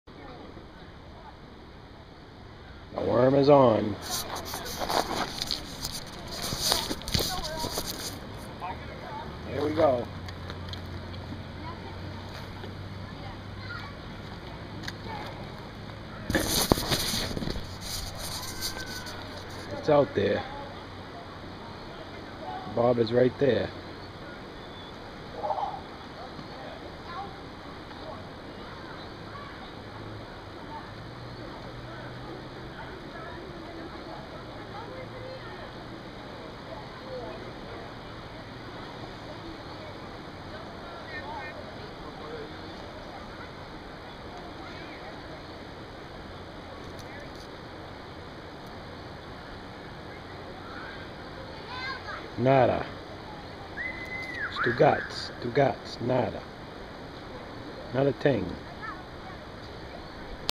Vlog 25...Soughegan River Merrimack New Hampshire
Swimming and fishing at river in merrimack nh,5/20/2012